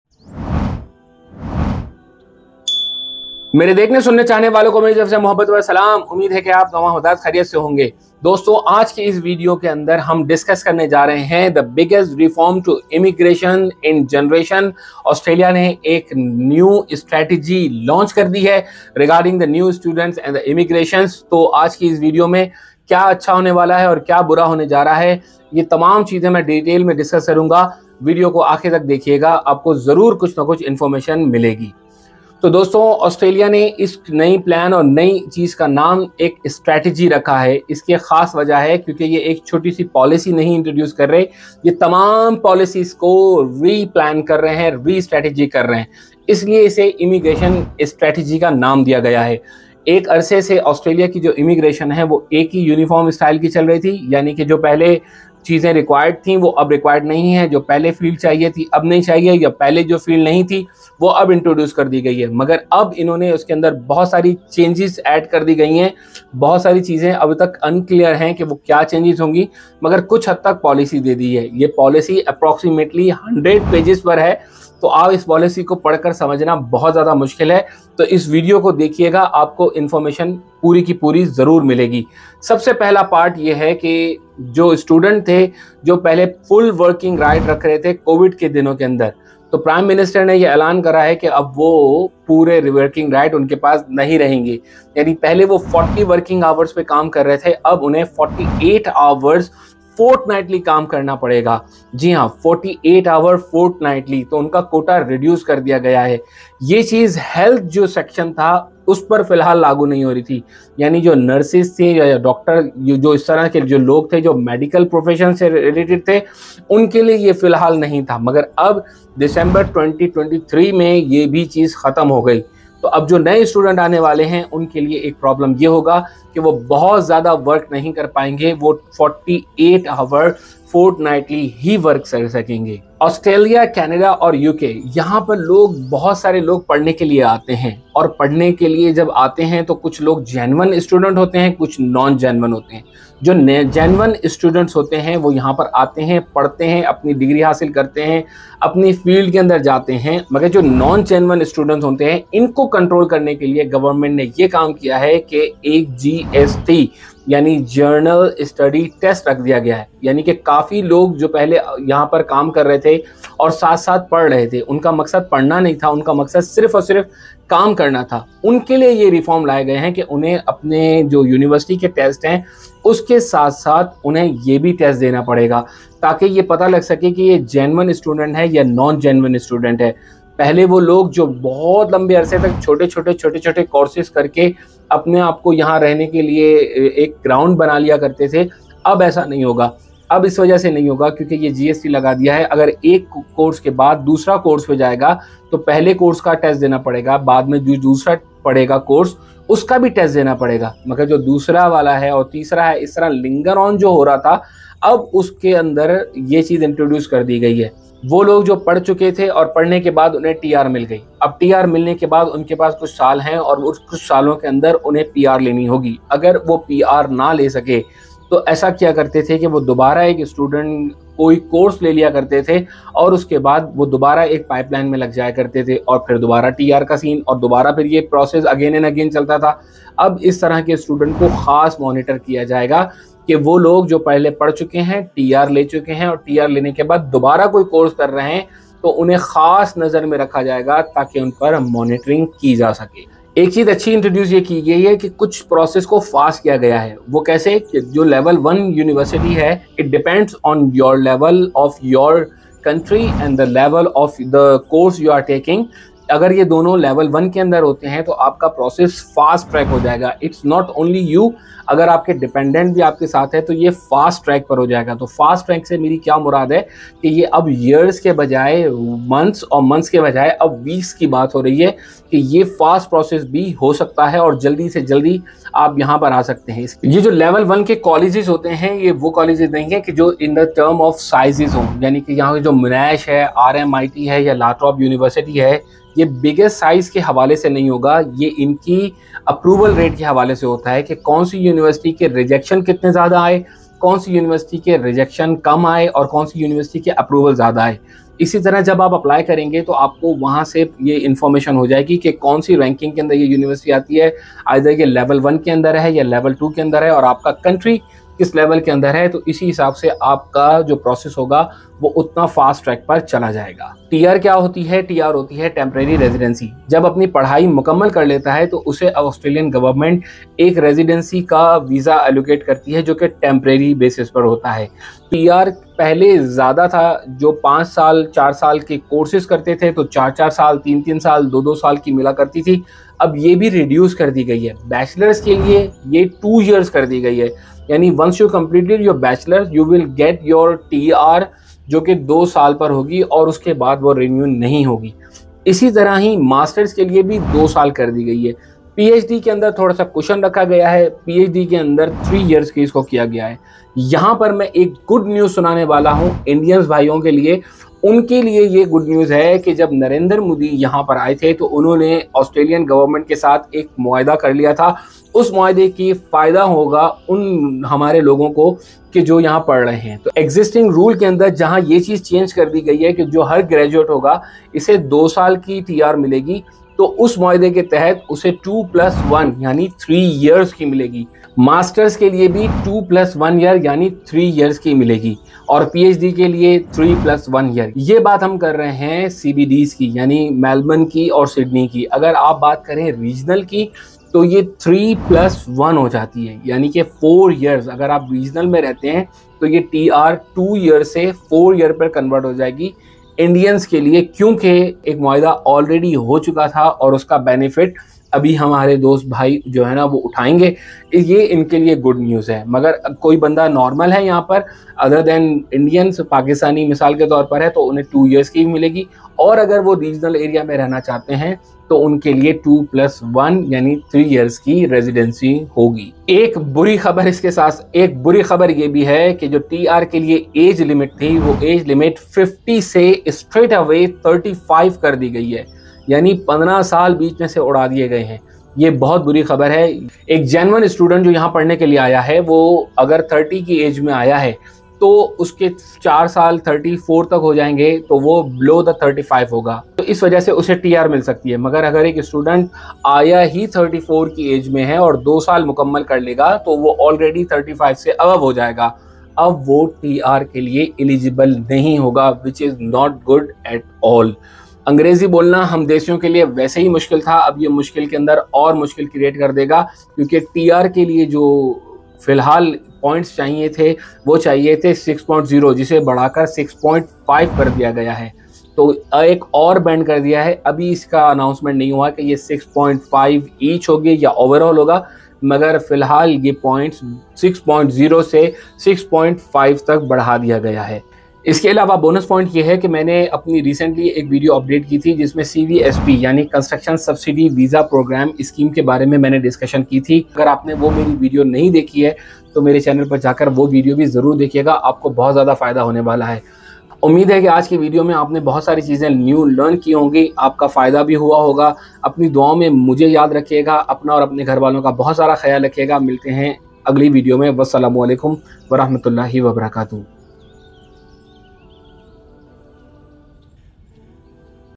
New Migration Strategy Australia In Detail 🇦🇺 | VISA Changes
Hi All, Hope all is well. Keep in mind, I am not a immigration agent or consultant however, I did my research to help our fellow.

In this video, I have described new immigration strategy Australia in detail. It is "the biggest reforms to migration in a generation".
The measures include:
Covid Concession Ends: Students will work 48 hours per fortnight
GST (General Student Test) is introducing
Additional scrutiny for student who takes student course again n again
Increasing minimum English language
TR graduate visa reducing
List 1 College visa is going to be fast tracked
TR years reducing
Good news for Indian in regards to TR